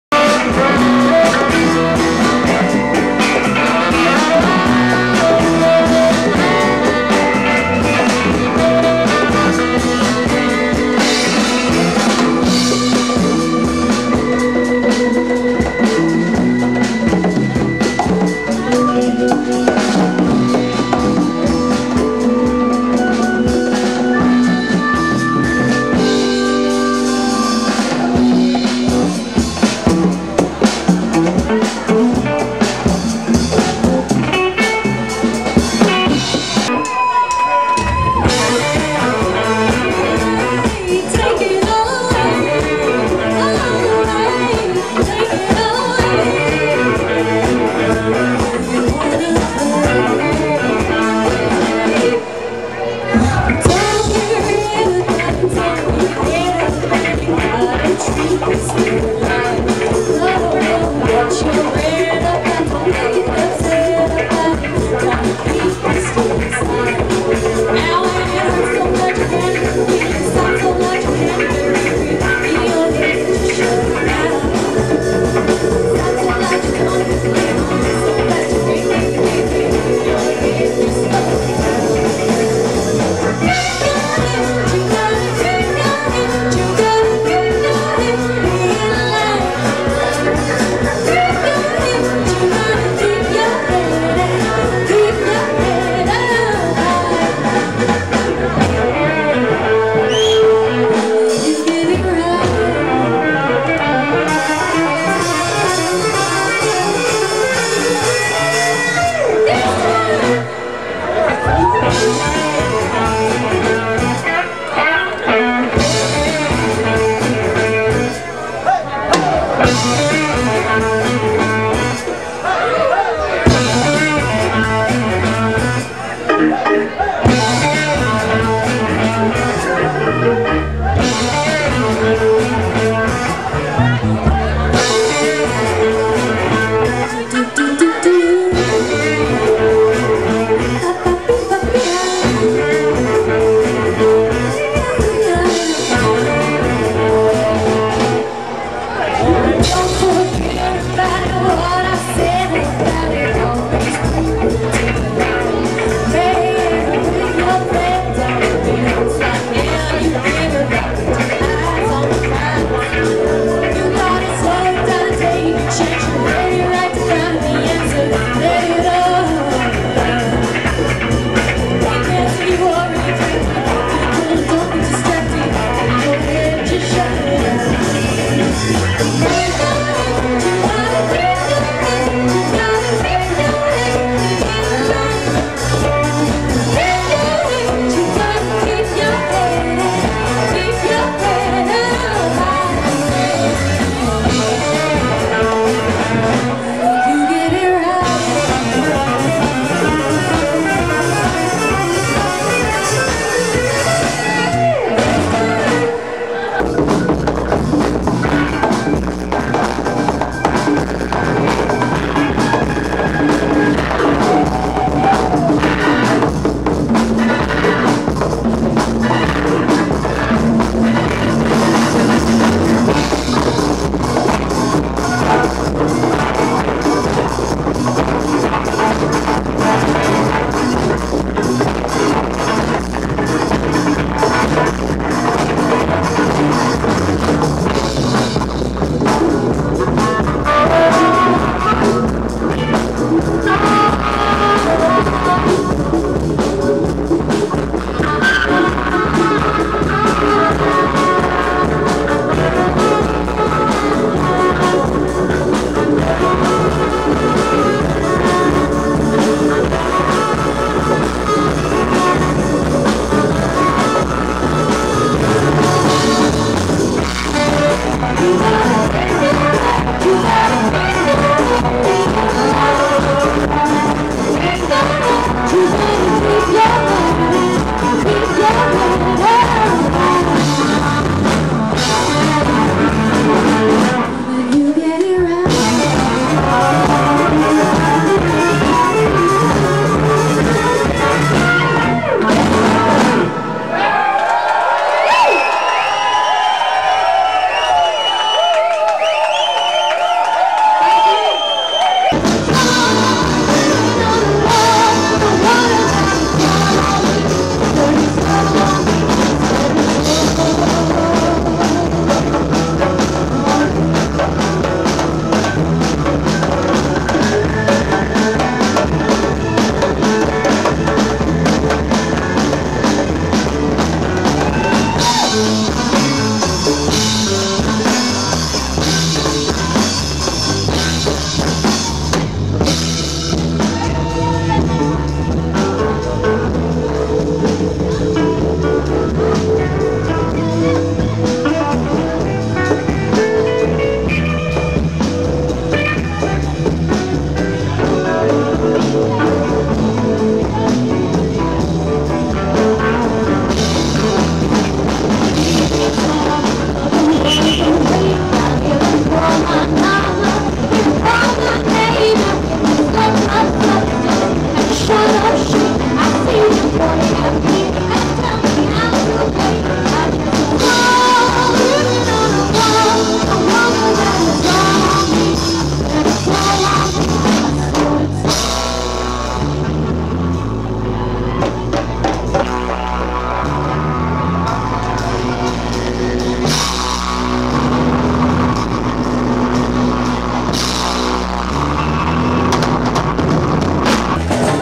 Jahm Band @Jigs and rigs 10.wmv
The Jahm Band playing at the Jigs and Rigs festival 2010 on Rathlin Island